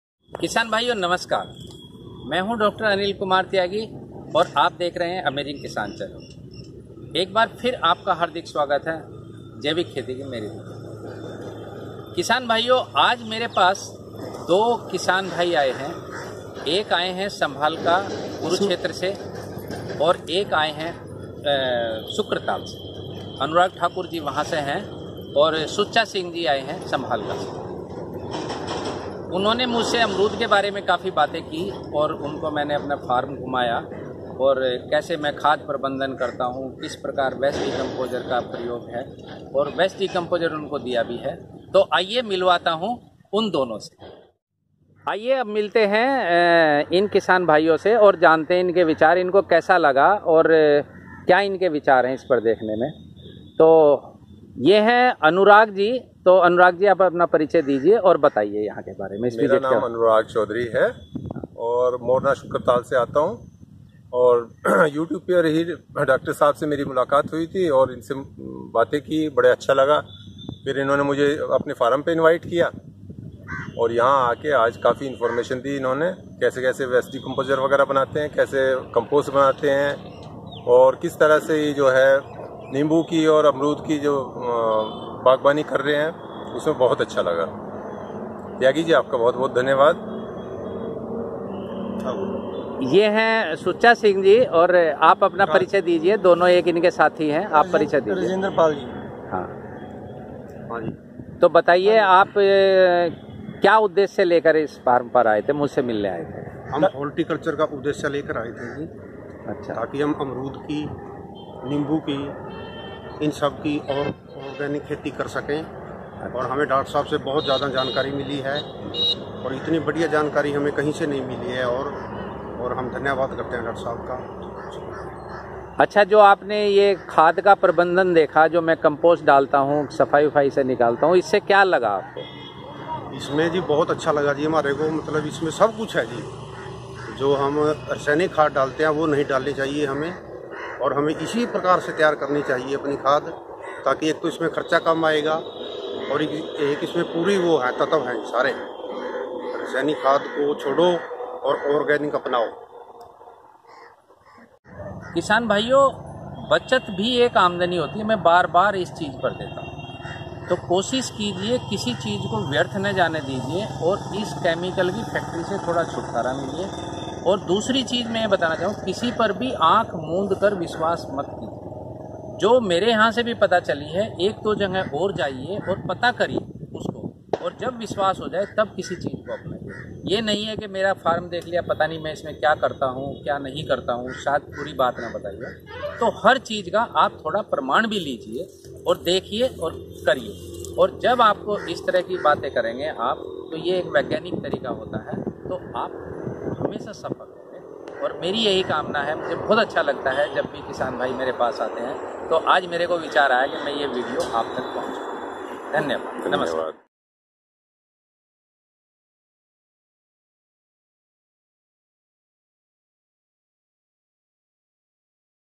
50. अमेजिंग किसान फार्म पर कुछ किसान भाई - 1#AmazingKisan
अमेजिंगकिसान फार्म पर आज दिनांक 16 मार्च 2019 को श्री सुच्चा सिंह, श्री राजेंद्र पाल, सम्भालकी, कुरुक्षेत्र और डॉ अनुराग चौधरी, मोरना, शुक्रताल, मुज़फ्फरनगर से आये। इस वीडियो में उनसे मुलाकात दिखाई गई है।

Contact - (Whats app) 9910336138